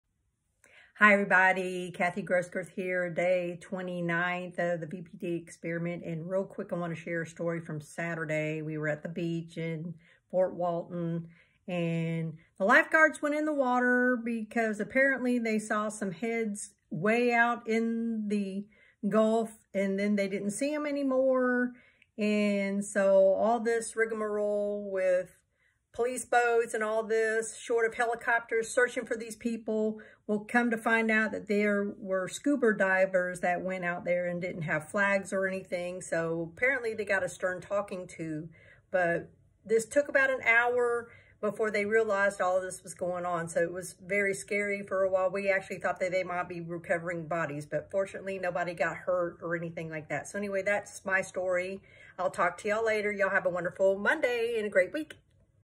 VPDExperiment Day 29 - My beach story from Saturday. 💦🤿⚠️
VPDExperiment Day 29 - I have an Interesting story from our day at the beach Saturday. 💦🤿⚠️ Anyhow I hope you all have a great Monday! ❤️